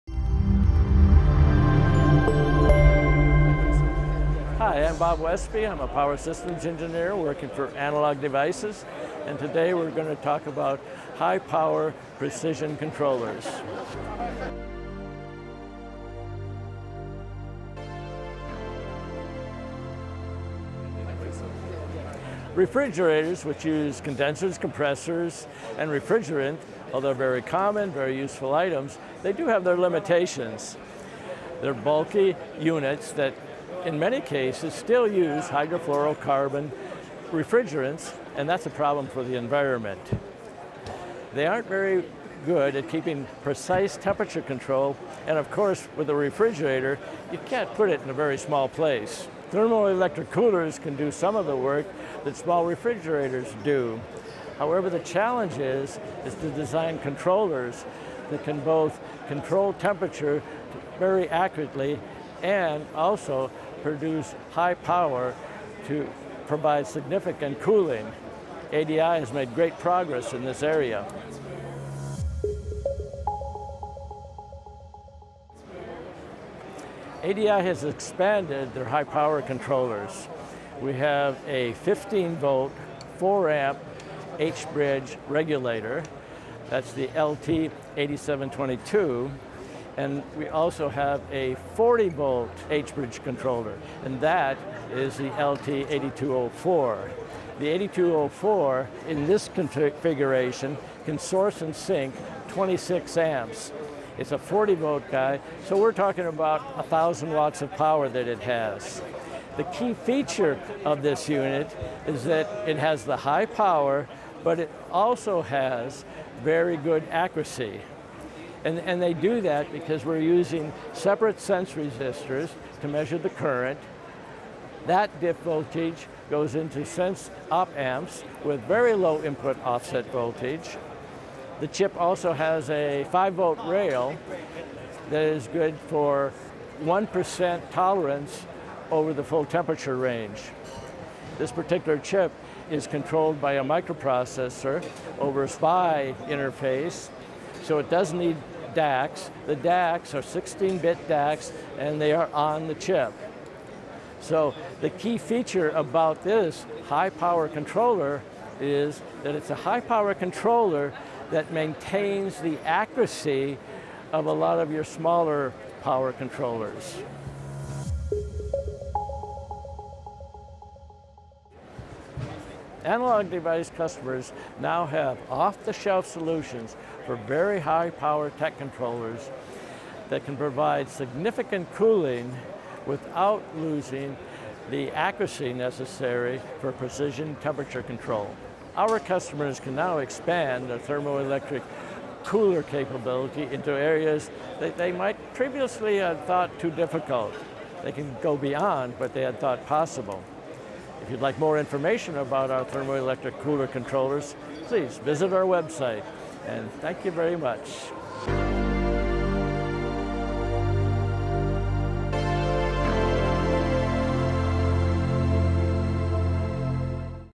High Current Precision Temperature Control Systems
Watch this video to see how ADI is expanding the power and capability of their thermoelectric coolers. Learn more about our controllers using a combination of linear and switcher power outputs in a patented technique to provide efficient TEC control with a single inductor.